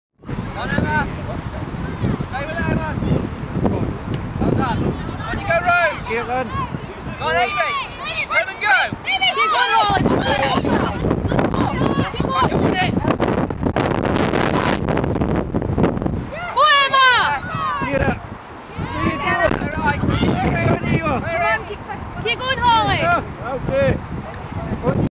13s v Legends - final league match 3
Under 13s attack is broken down and transformed into a Legends attack.